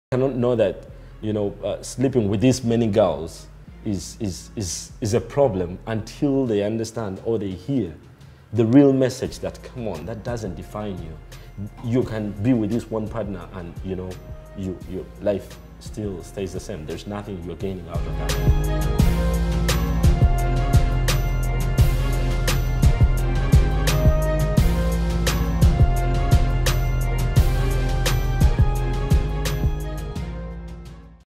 “Sleeping with many girls does not define you” Godfrey Kuteesa on CedricLiveShow
Cedric speaks with Godfrey Kuteesa on the foundation he has set up to support the BOYCHILD